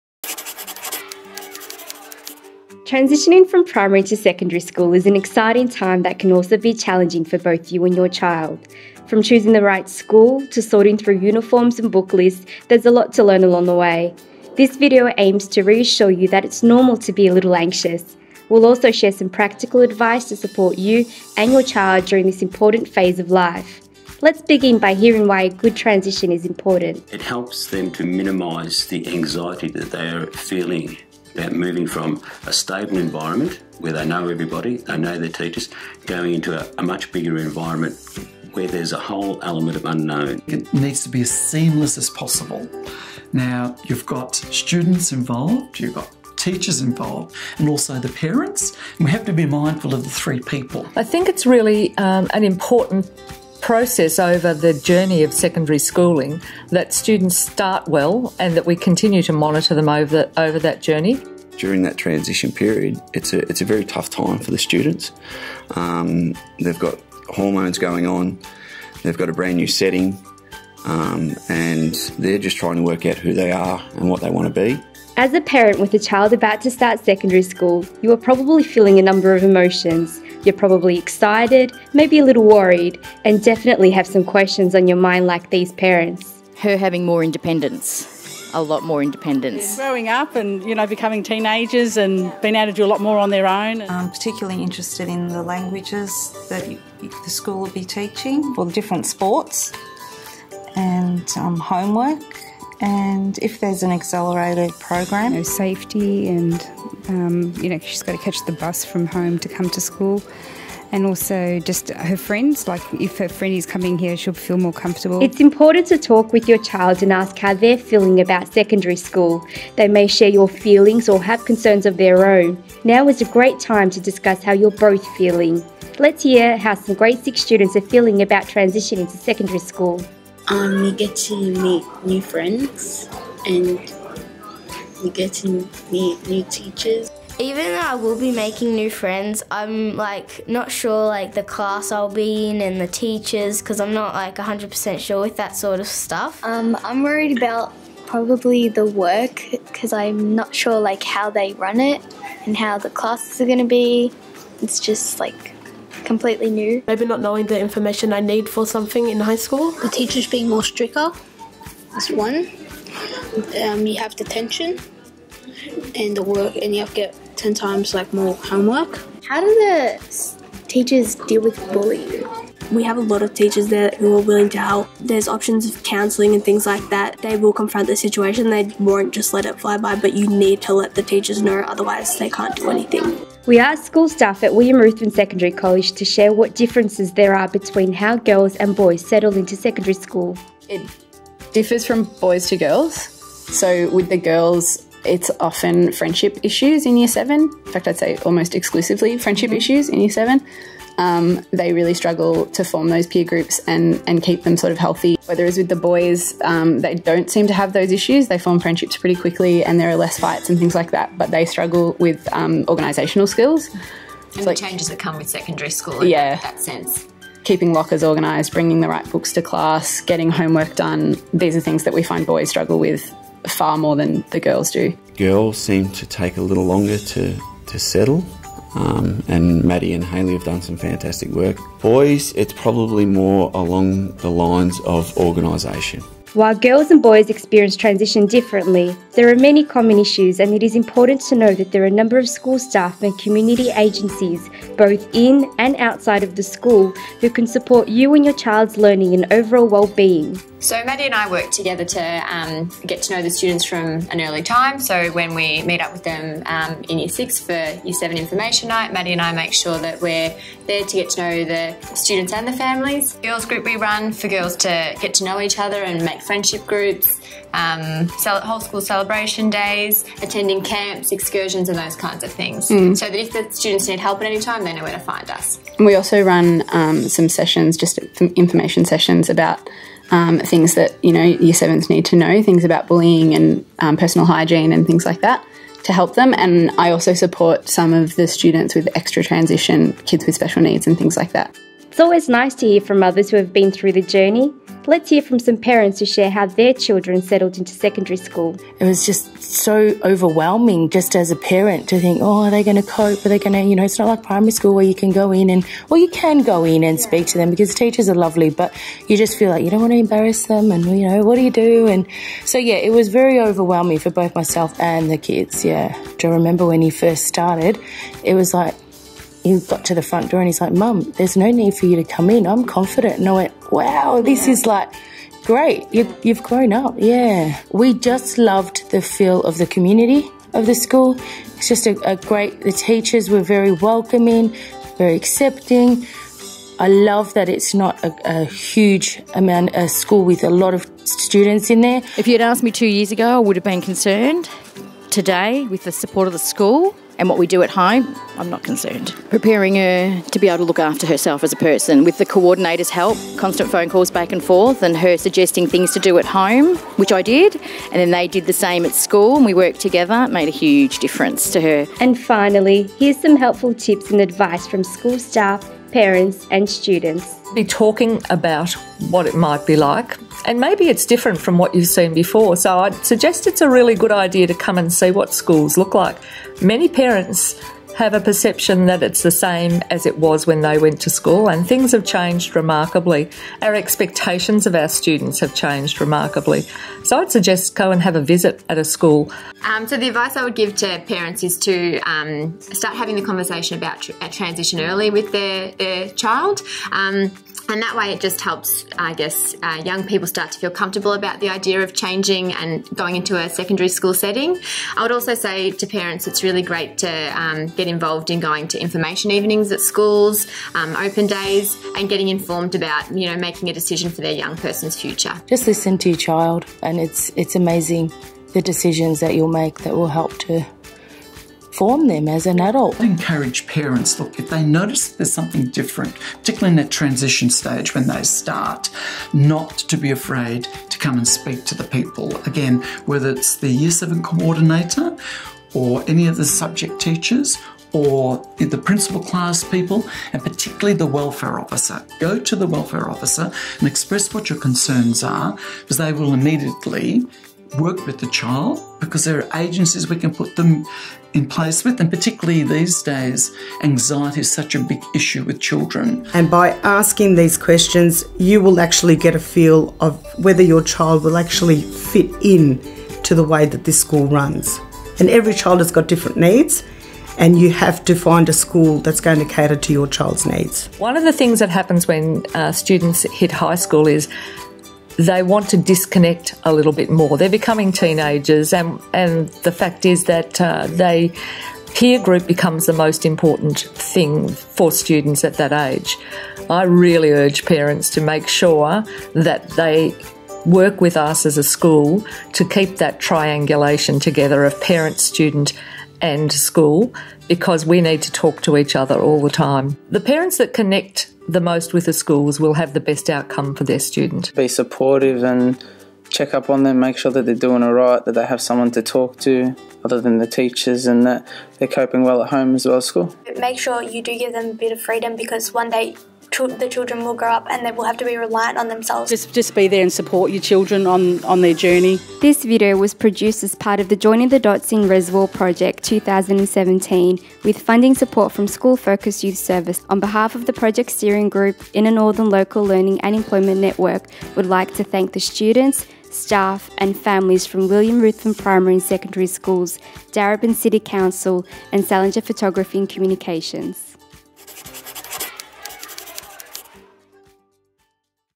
Joining the Dots Reservoir: Parent Digital Case Study
This parent-focused digital case study, which is part of the 2017 Joining the Dots Reservoir project, is intended as a guide to assist parents of children at William Ruthven Secondary College in understanding the importance of school transitions and what their children may be experiencing as they journey into and through secondary school.

A successful transition from primary to secondary school is a key factor in effective engagement in learning. Joining the Dots takes the transition mindset beyond the administrative transfer of students from Year 6 to Year 7, to a developmental journey from Year 5 to 9 for students and their families, as they settle in and manage themselves and their learning in a secondary environment.

Based on the William Ruthven Secondary College experience there are many valuable lessons for parents whose children are at other schools.